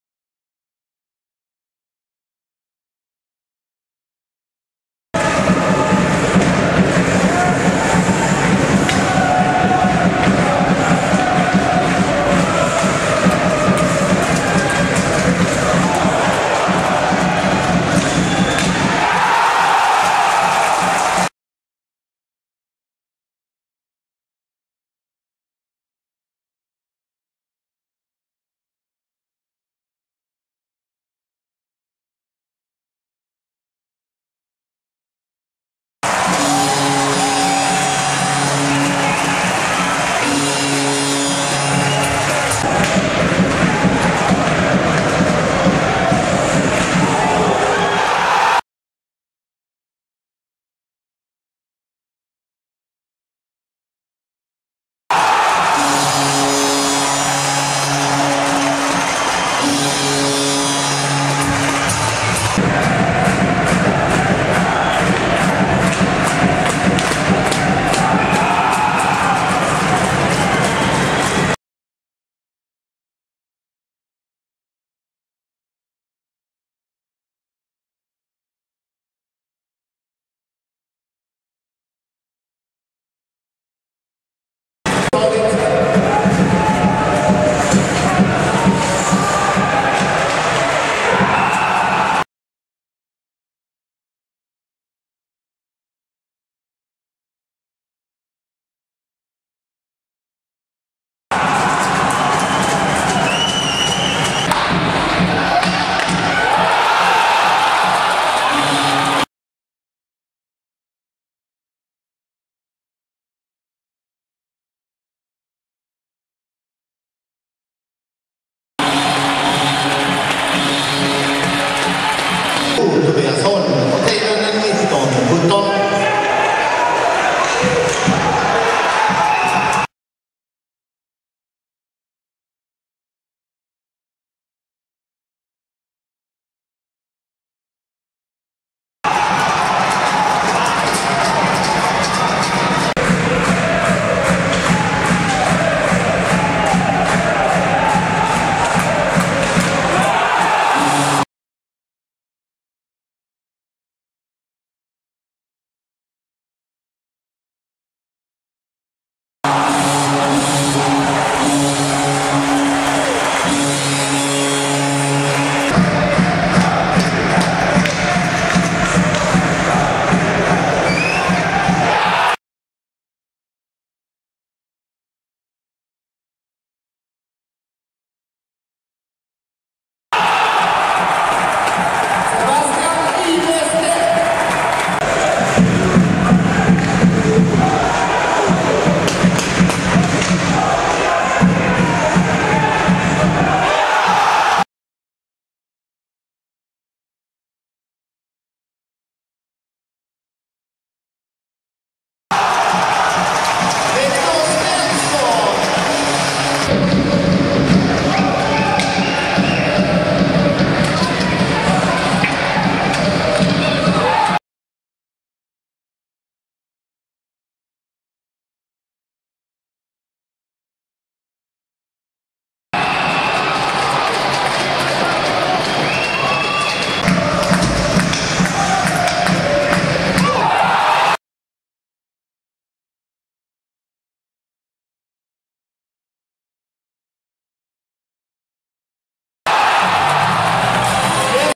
KRIF - OIK 7-4
KRIF Hockey - Olofströms IK 7-4 3:an, mot 1:an i serien. Och vilken målkavalkad lagen bjöd på